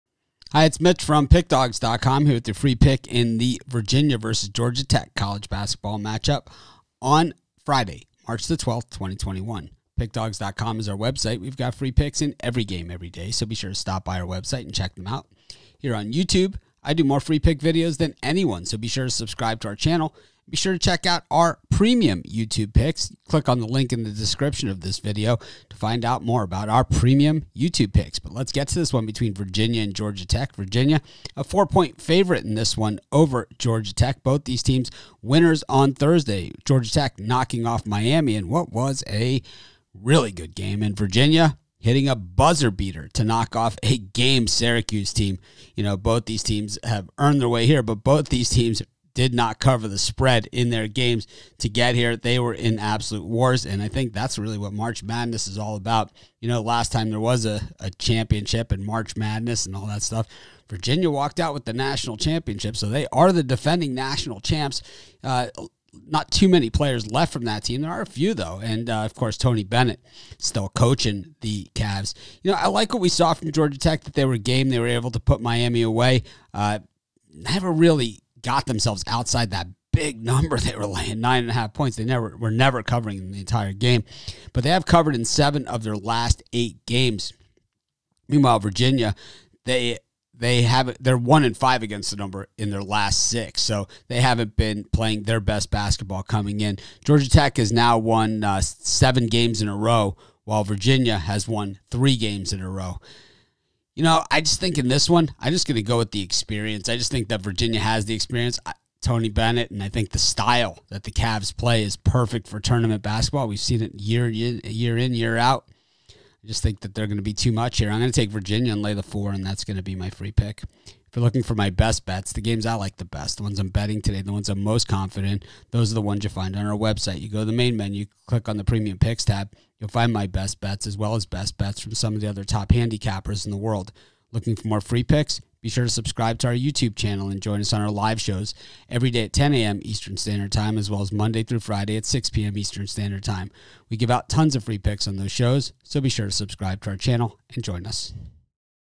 Virginia vs Georgia Tech 3/12/21 Free College Basketball Pick and Prediction CBB Betting Tips
The Georgia Tech Yellowjackets take on the Virginia Cavaliers in Friday college basketball action in the annual March Madness NCAA Tournament. Get Mitch's College Basketball Pick and Prediction for this game.